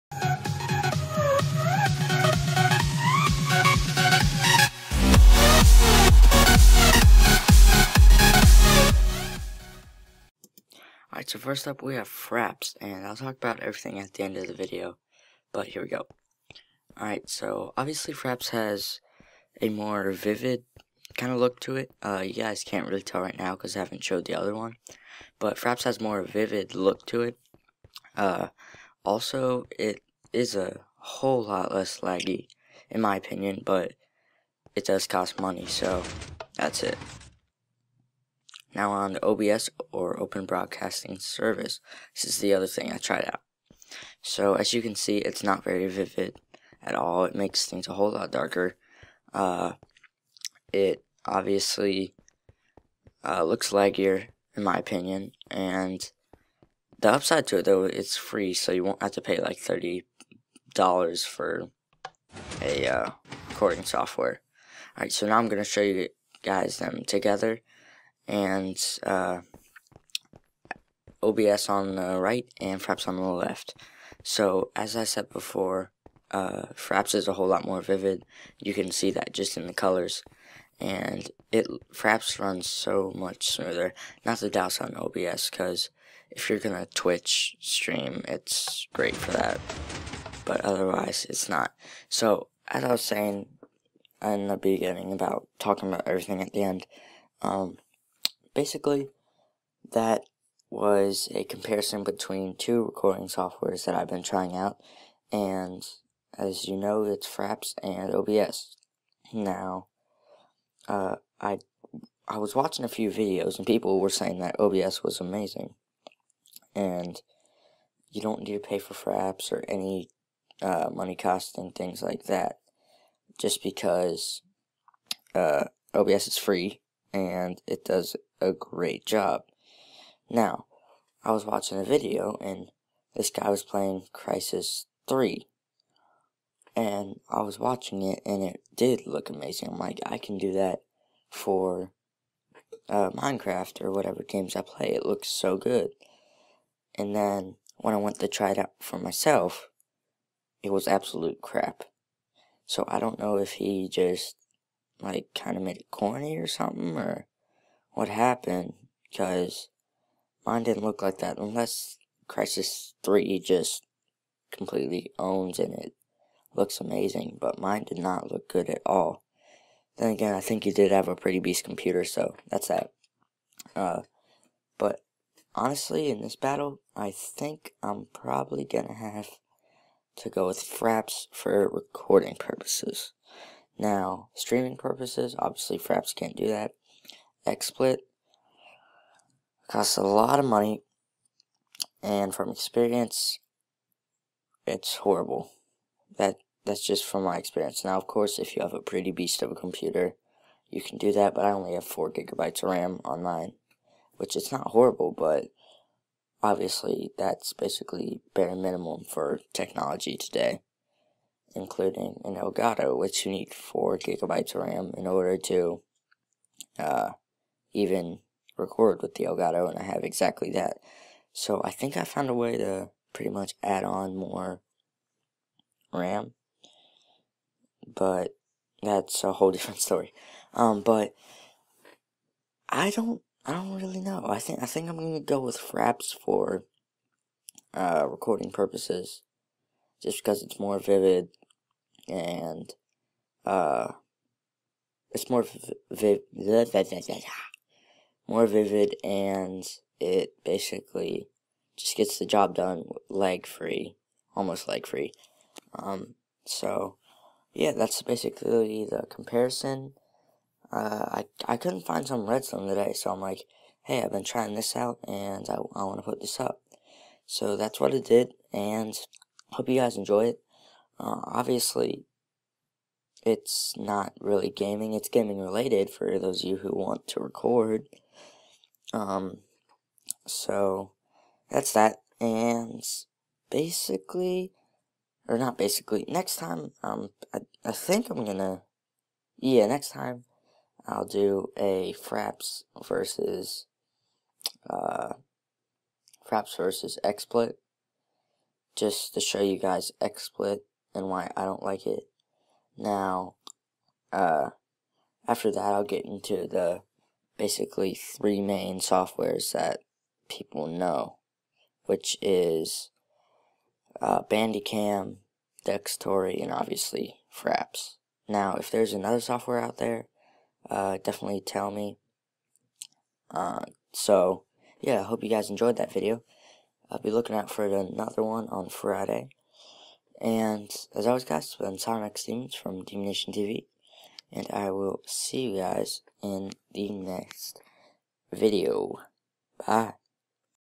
Fraps vs OBS-WHO WINS?!?
Hey guys Demonz here,and today I am here with a comparison video between two recording soft-wares: Fraps and OBS.